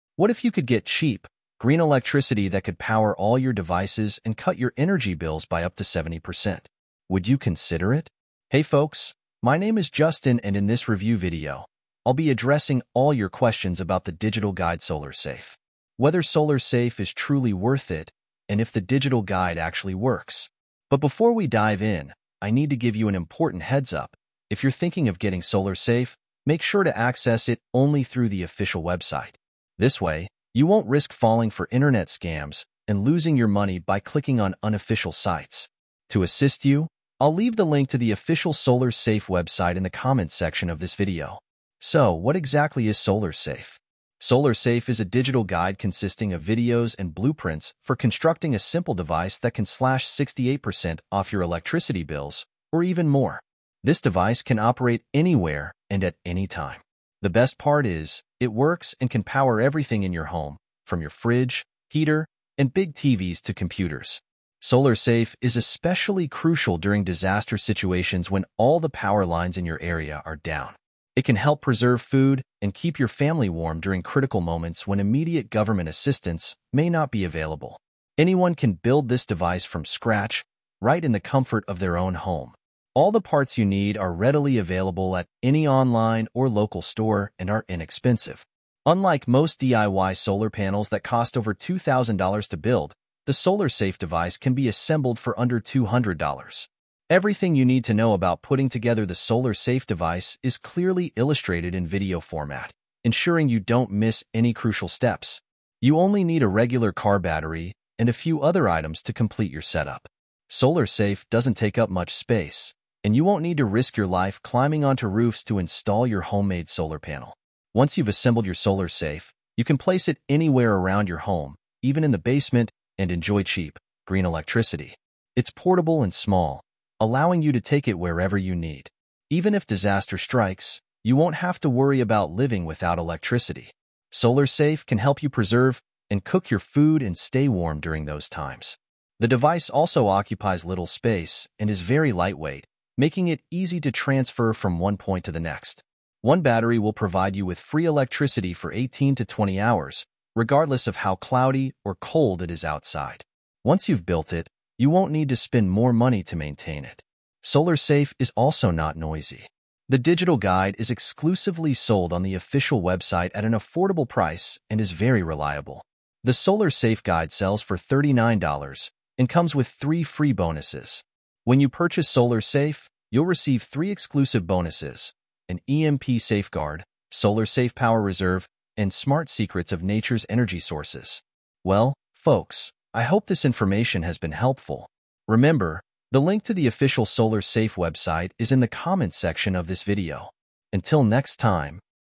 SOLAR SAFE REVIEW (❌IMPORTANT ALERT 2024❌) The Digital Guide Solar Safe Works? SOLAR SAFE WEBSITE
What if you could get cheap, green electricity that could power all your devices and cut your energy bills by up to 70%? Would you consider it? Hey folks, my name is Justin and in this review video, I'll be addressing all your questions about the digital guide Solar Safe. Whether Solar Safe is truly worth it and if the digital guide actually works. But before we dive in, I need to give you an important heads-up: if you're thinking of getting Solar Safe, make sure to access it only through the official website. This way, you won't risk falling for internet scams and losing your money by clicking on unofficial sites. To assist you, I'll leave the link to the official Solar Safe website in the comments section of this video.

So, what exactly is Solar Safe? This device can operate anywhere and at any time. The best part is, it works and can power everything in your home, from your fridge, heater, and big TVs to computers. Solar Safe is especially crucial during disaster situations when all the power lines in your area are down. It can help preserve food and keep your family warm during critical moments when immediate government assistance may not be available.

Anyone can build this device from scratch, right in the comfort of their own home. All the parts you need are readily available at any online or local store and are inexpensive. Unlike most DIY solar panels that cost over $2,000 to build, the Solar Safe device can be assembled for under $200. Everything you need to know about putting together the Solar Safe device is clearly illustrated in video format, ensuring you don't miss any crucial steps. You only need a regular car battery and a few other items to complete your setup.

Solar Safe doesn't take up much space, and you won't need to risk your life climbing onto roofs to install your homemade solar panel. Once you've assembled your Solar Safe, you can place it anywhere around your home, even in the basement, and enjoy cheap, green electricity. It's portable and small, allowing you to take it wherever you need. Even if disaster strikes, you won't have to worry about living without electricity. Solar Safe can help you preserve and cook your food and stay warm during those times. The device also occupies little space and is very lightweight, making it easy to transfer from one point to the next.

One battery will provide you with free electricity for 18 to 20 hours, regardless of how cloudy or cold it is outside. Once you've built it, you won't need to spend more money to maintain it. Solar Safe is also not noisy. The digital guide is exclusively sold on the official website at an affordable price and is very reliable. The Solar Safe guide sells for $39 and comes with three free bonuses. When you purchase Solar Safe, you'll receive three exclusive bonuses: an EMP Safeguard, Solar Safe Power Reserve, and Smart Secrets of Nature's Energy Sources.

Well, folks, I hope this information has been helpful. Remember, the link to the official Solar Safe website is in the comments section of this video. Until next time!